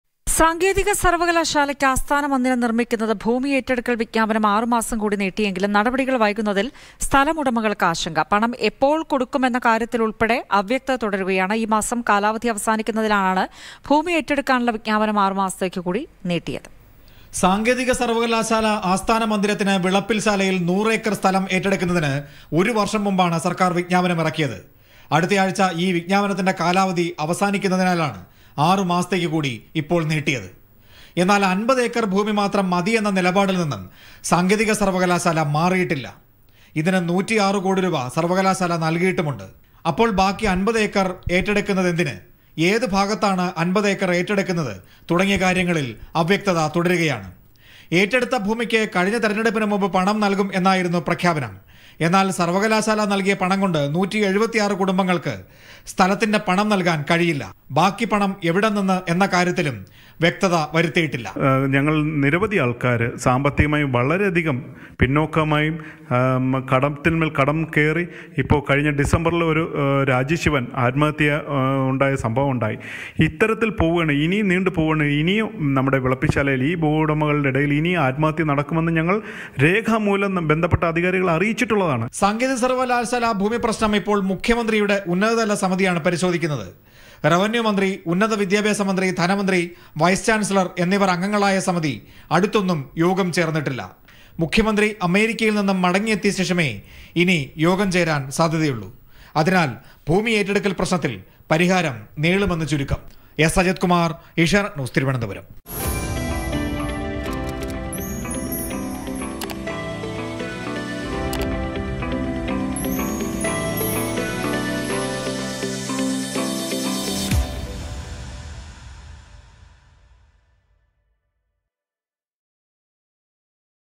സാങ്കേതിക സർവകലാശാല ആസ്ഥാന മന്ദിരം: ഭൂമി ഏറ്റെടുക്കൽ വിജ്ഞാപനം 6 മാസം കൂടി നീട്ടി | Land Acquisition
സാങ്കേതിക സർവകലാശാല ആസ്ഥാന മന്ദിരം: ഭൂമി ഏറ്റെടുക്കൽ വിജ്ഞാപനം 6 മാസം കൂടി നീട്ടി, പണം എപ്പോൾ കൊടുക്കുമെന്ന കാര്യത്തിലുൾപ്പടെ അവ്യക്തത. നടപടികൾ വൈകുന്നതിൽ സ്ഥലയുടമകൾക്ക് ആശങ്ക
Land acquisition for Technological university: acquisition notification extended for another six months raising concerns among landowners.